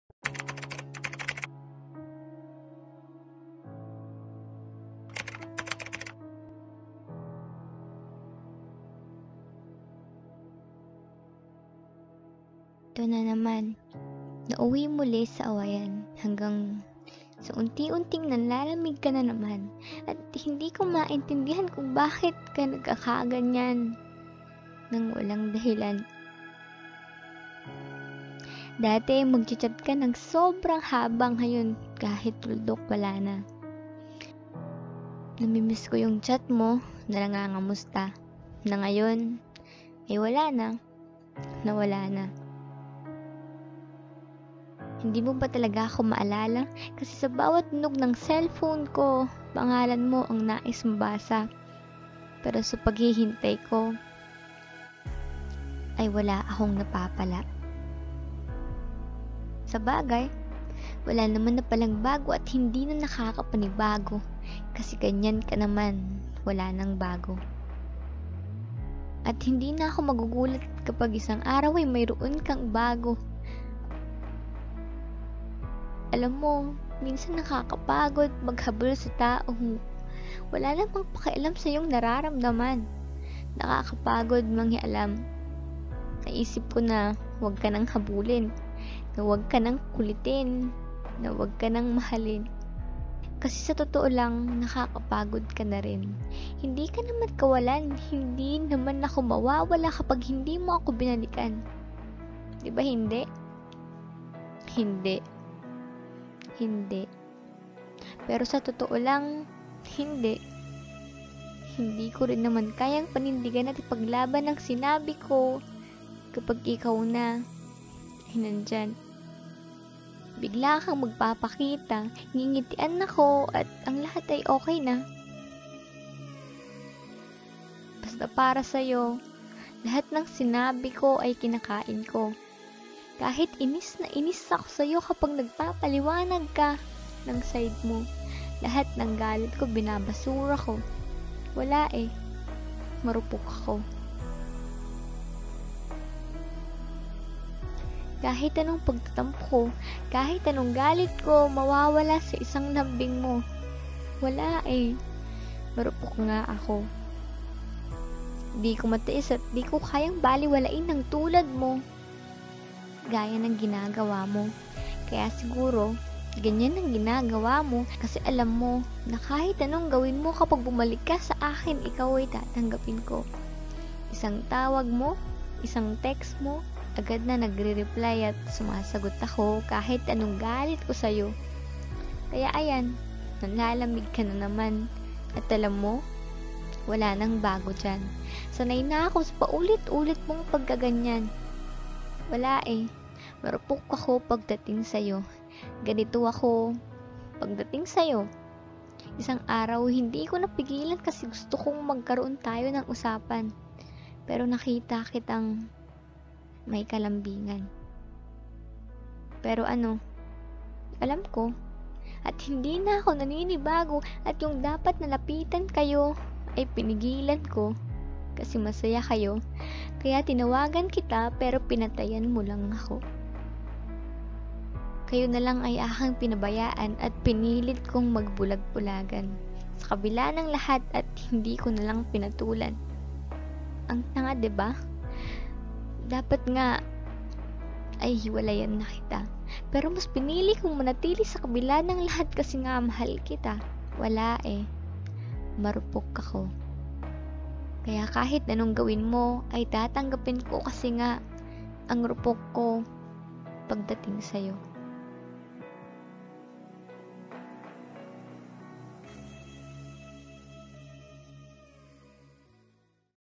Marupok (TagalogSpokenPoetry)
MARUPOK...wala eh marupok nga kasi. hahhaah char

Writer: JellaiSarmiento
Voice over: Jellai Sarmiento

HELLO FRIENDS

Comment your Suggested TOPIC

THANKS